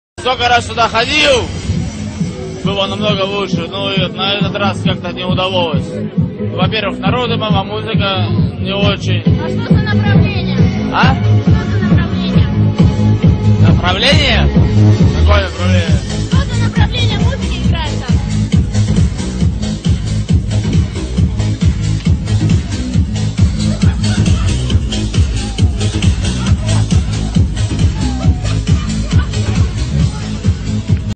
Russian clubber, Dimitri's reaction to the SotM11 video contest. (subtitled)
Turn subtitles (close captioning) on by pressing the 'CC' button at the bottom of the video.
A Russian clubber is interviewed to his reaction to the SotM11 Video Contest.
following two video entries.